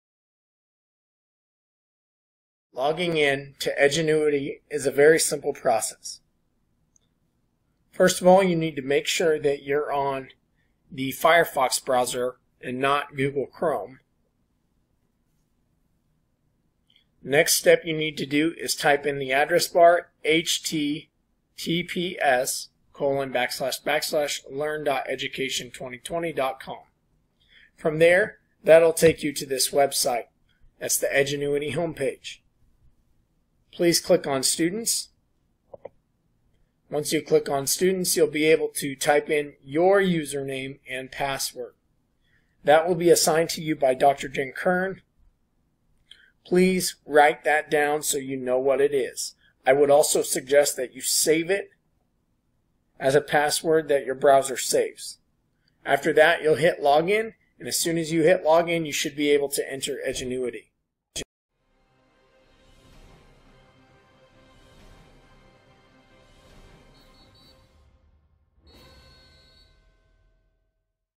Logging into Edgenuity
This is for Summer Students who are unaware how to log into Edgenuity.